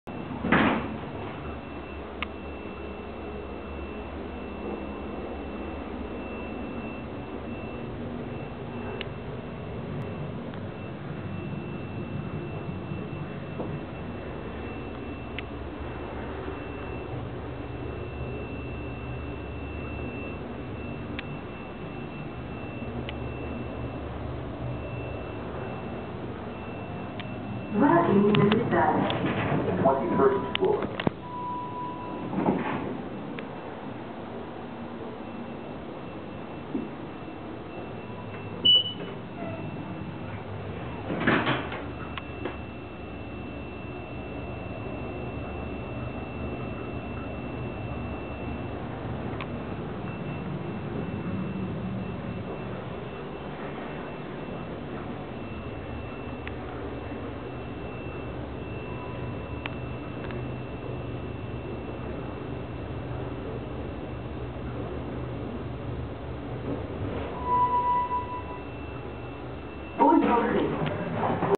Montgomery traction elevator - Delta Montreal Hotel
Nice Montgomery elevators at the Delta Montreal. Opened in 1986.